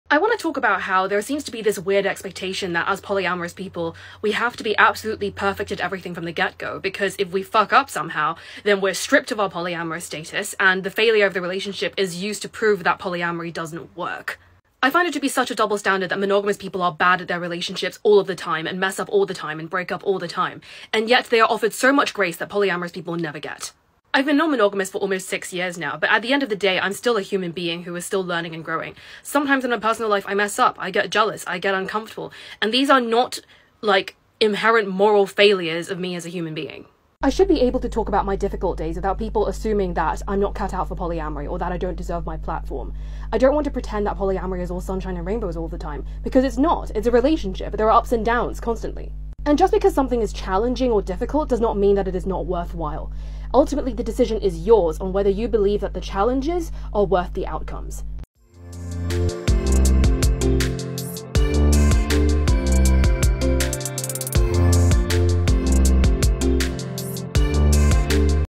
We Don't Talk About This
my thoughts on polyamorous perfectionism.